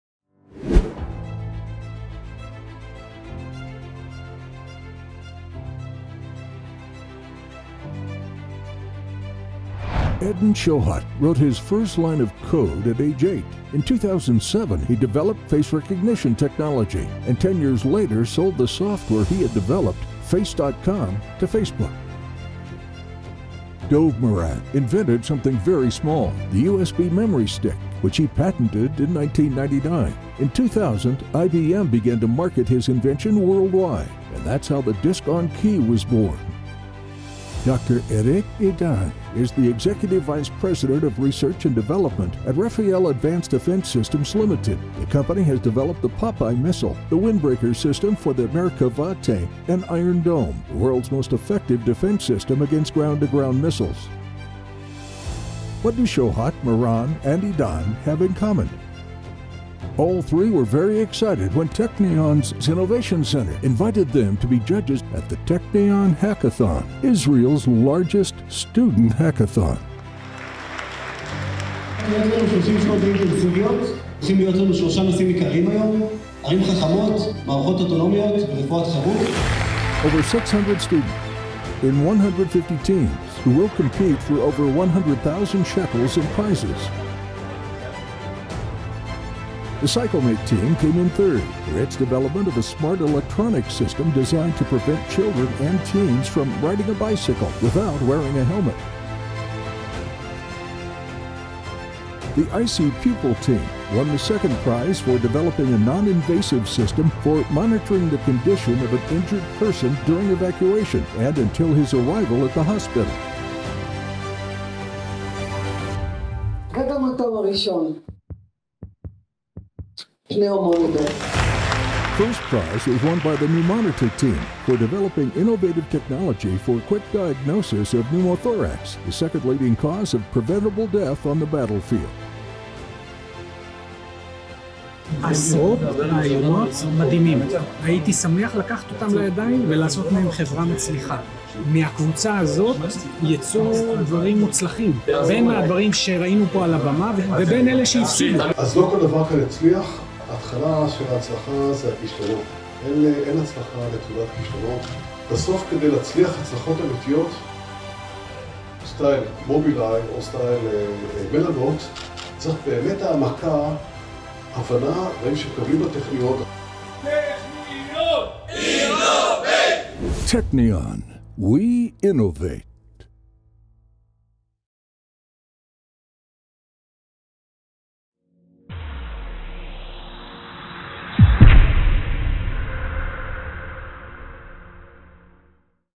Technion: We Innovate
We Innovate!
 t-hub - The Technion Innovation Center presents:
The biggest hatckathon competition in Israel!

Over 600 students!
in 150 teams,
who compete for over NIS 100,000 in prizes!

Eden Shochat
Dov Moran
Dr. Irit Idan
Isaac Bentwich M.D
Amir David Nissan Cohen

PneuMonitor
ICPupil
Cyclmate

הפקה: עושים עניין בע"מ
צילום: מאור כהן
במאי: אבי שמול